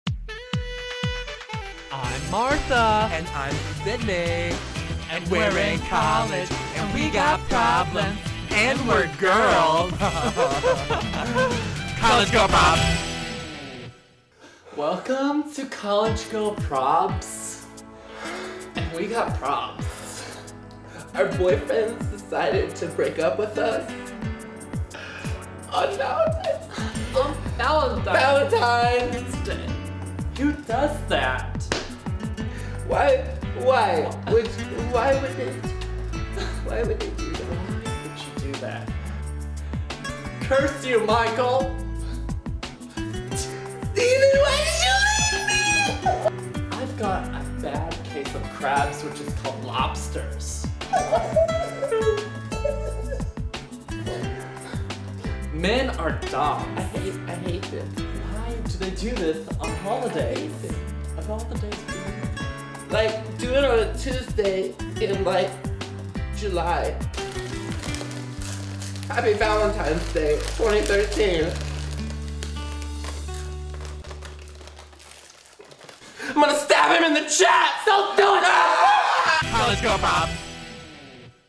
College Gurl Probz: Valentines Day
Guys point of view when college gurls complain and gossip about their terrible/not so terrible lives